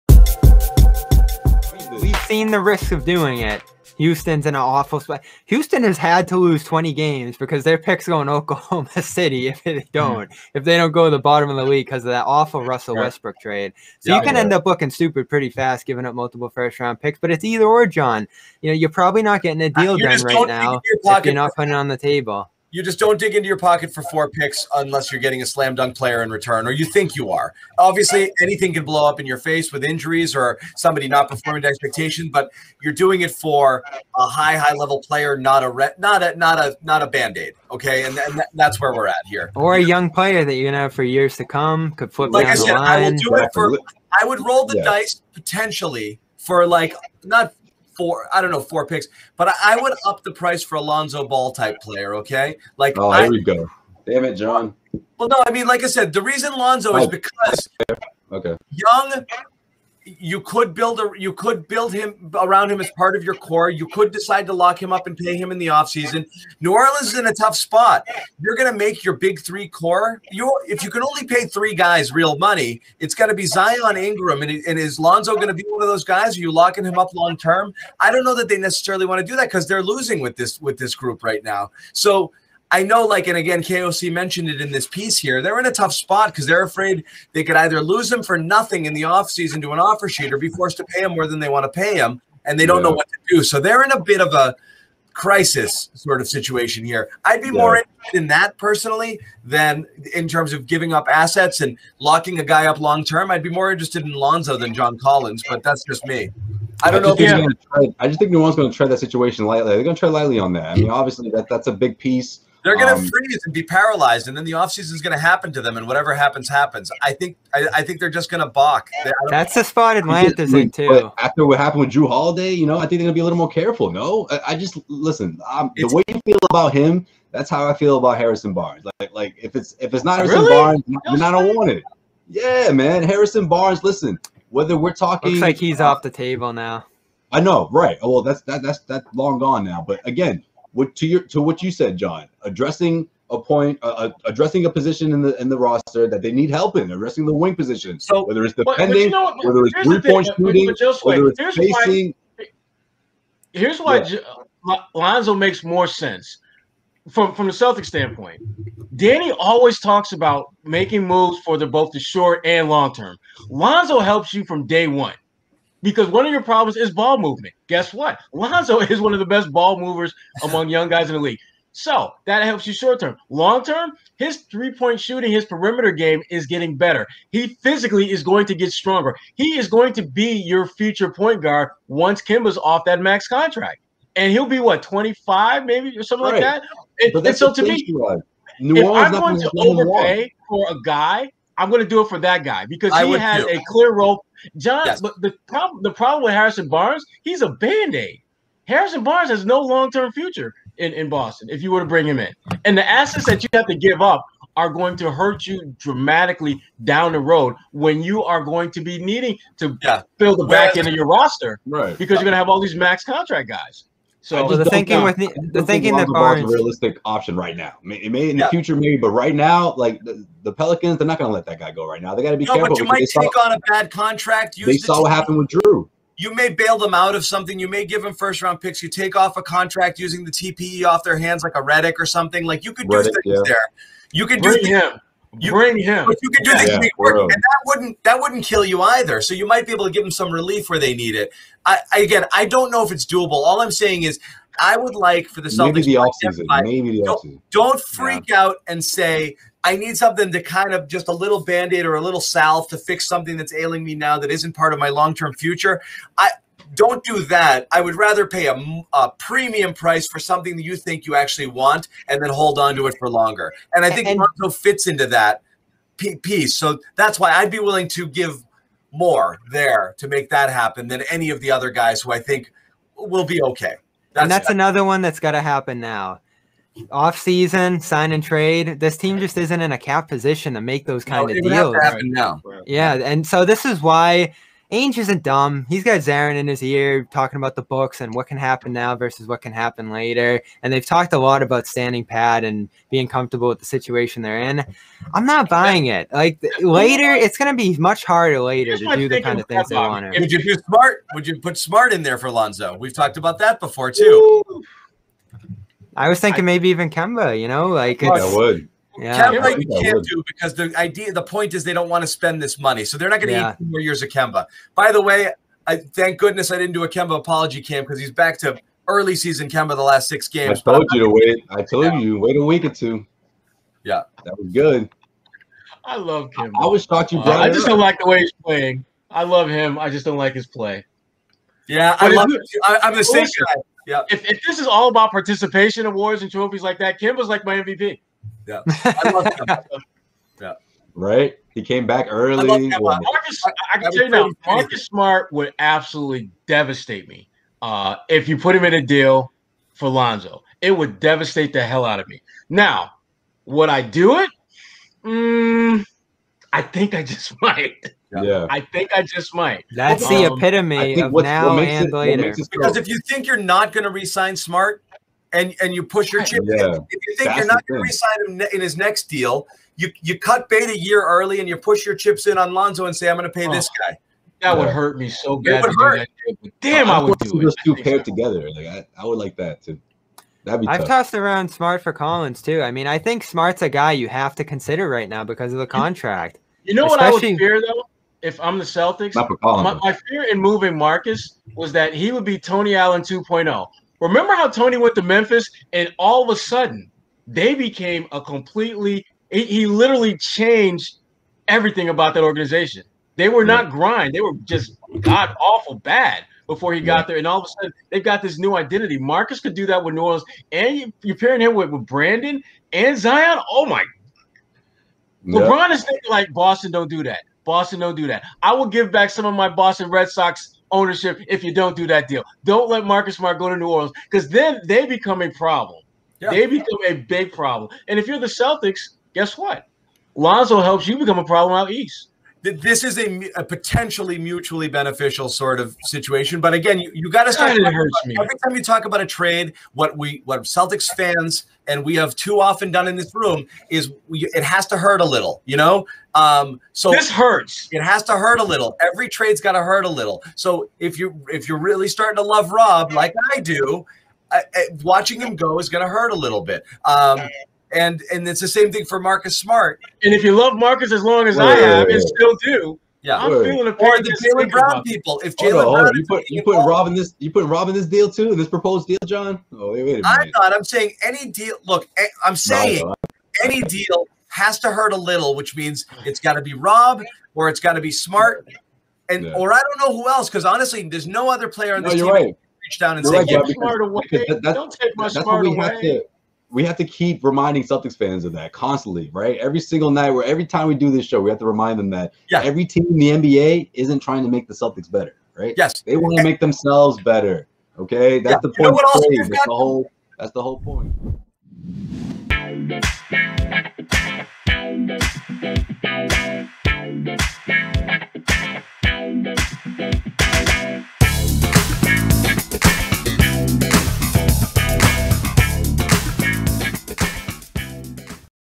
Should Lonzo Ball Be Celtics Top Trade Target?
With the trade deadline speeding forward on March 25th, multiple players have hit the trade block, one of which is Pelicans point guard Lonzo Ball. As a gifted passer with a knack for defense, what would Lonzo Ball's potential fit on Boston be? What would be the asking price? The Garden Report weighs in.